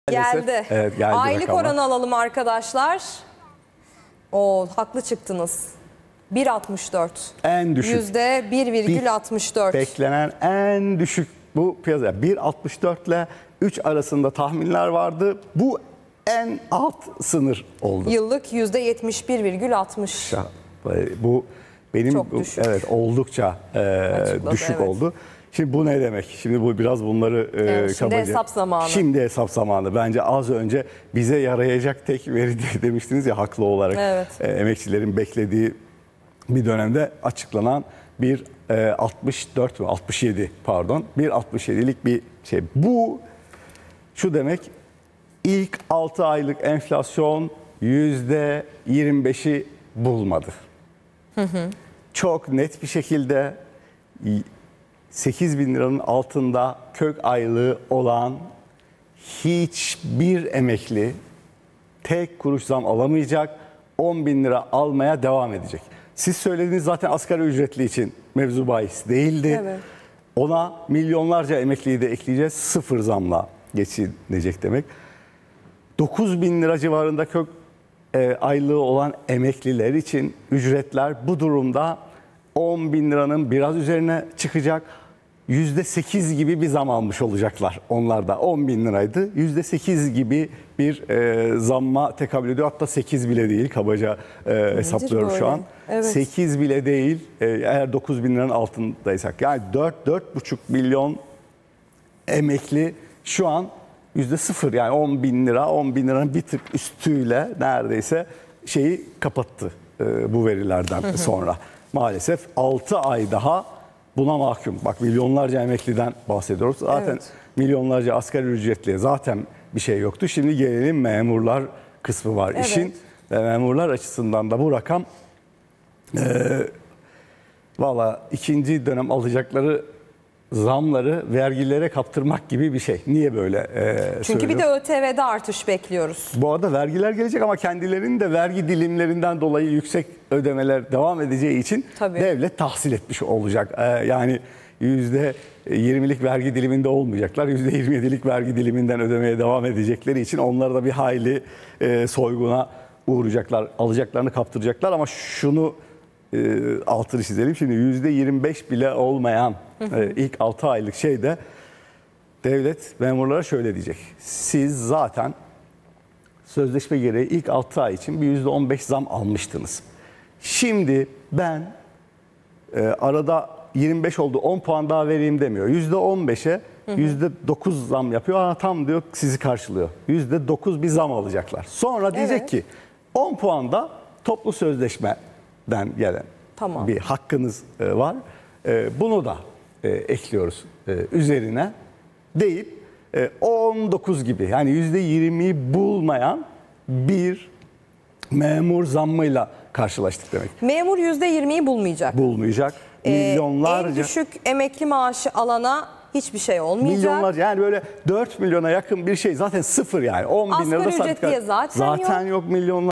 SON DAKİKA! Memur ve emeklinin beklediği haber geldi! Haziran ayı enflasyonu açıklandı!
TÜİK'e göre haziranda enflasyon yıllık yüzde 71,60, altı aylık yüzde 24,73, aylık yüzde 1,64 oldu. ENAG ise yıllık enflasyonu yüzde 113,08, altı aylık enflasyonu yüzde 41,16, aylık enflasyonu yüzde 4,27 hesapladı.